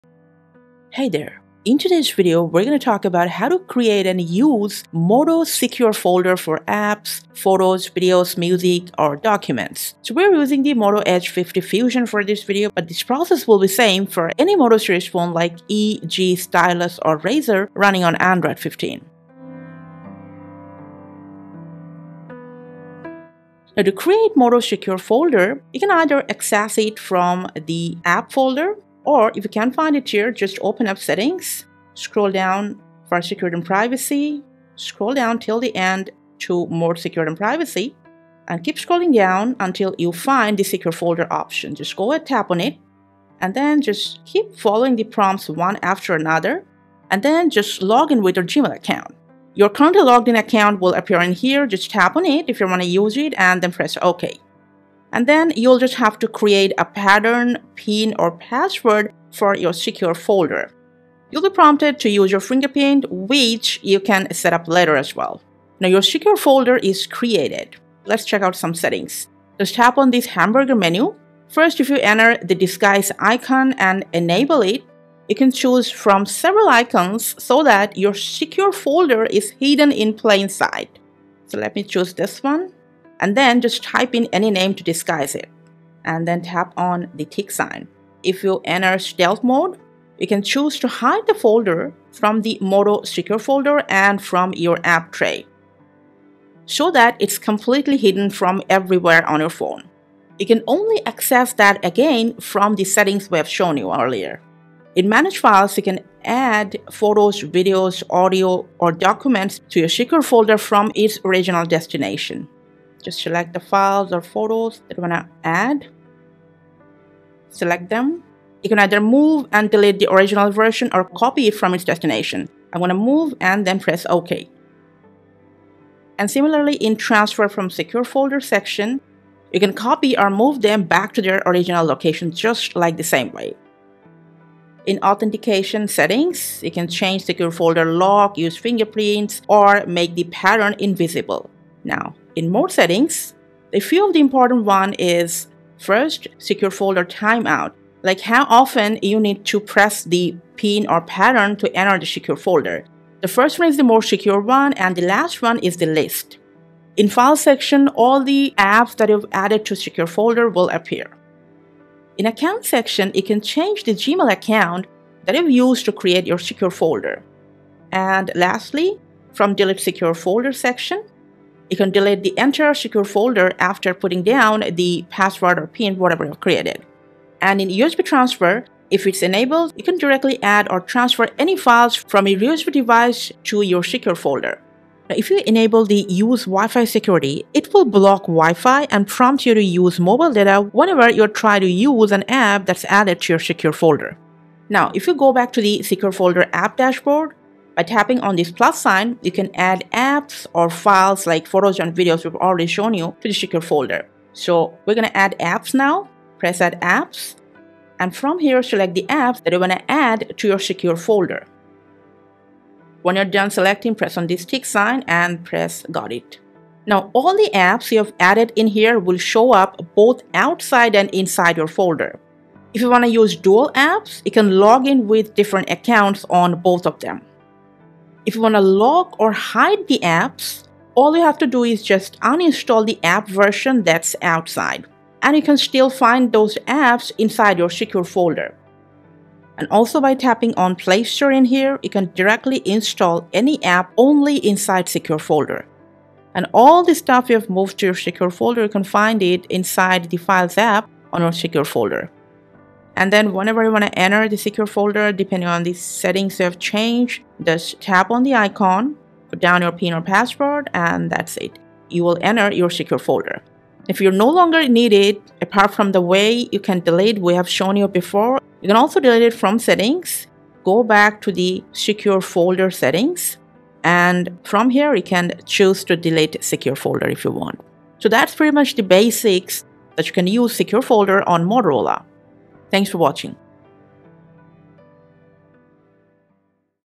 Moto Edge: How To Use Secure Folder on Motorola!
Confused about how to use the Secure Folder on Motorola Edge 50 Fusion or any other Motorola Phone running Android 15, including Moto G, E, Stylus, or Razr series, to hide or lock Photos/ Videos/ Audio/ Apps and more? No worries. The Fix369 Team brought you a complete beginner’s guide to setting up Secure Folder on the Motorola Phone and how to use it as well.

In this video, we will show you how to:

• Configure Secure Folder on Motorola Phone for the first time.
• Check and enable privacy settings on Motorola Secure Folder
• Move or Copy pictures, videos, audios, etc. to Secure folder from the original location
• Move or Copy pictures, videos, audios, etc. tothe  original location from the Secure Folder
• Add Apps on Secure Folder and What Happens after Adding Apps
• Setup Secure Folder Authentication and More settings
• 2 Ways to delete Secure Folder on Motorola Phone
• Enter Secure Folder from the Phone

0:00 Intro: Setup Secure Folder on Motorola Phone
0:23 Create Secure Folder on Motorola
1:08 Check Secure Folder Privacy Settings
1:45 Add Photos/Videos/Audios to Secure Folder or Transfer files to original Location
2:14 Secure Folder Authentication and other Settings
2:49 Delete Secure Folder from the Secure Folder Settings
3:16 Add Apps to Secure Folder
4:12 Enter Secure Folder on Motorola Phone
4:25 Delete Secure Folder From Settings